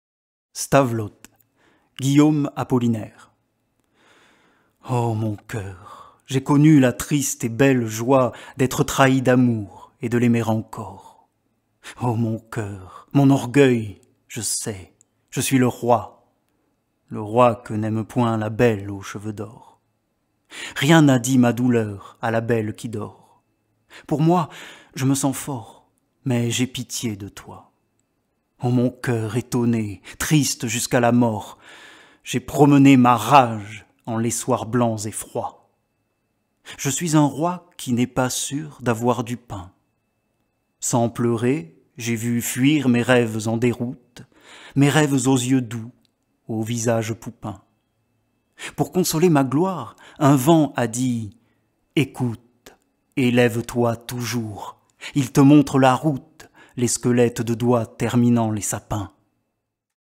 Apollinaire (Guillaume) STAVELOT - Ô mon cœur j'ai connu la triste et belle joie
Stavelot | Poème écrit par Guillaume Apollinaire (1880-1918)
Extrait du recueil Œuvres Complètes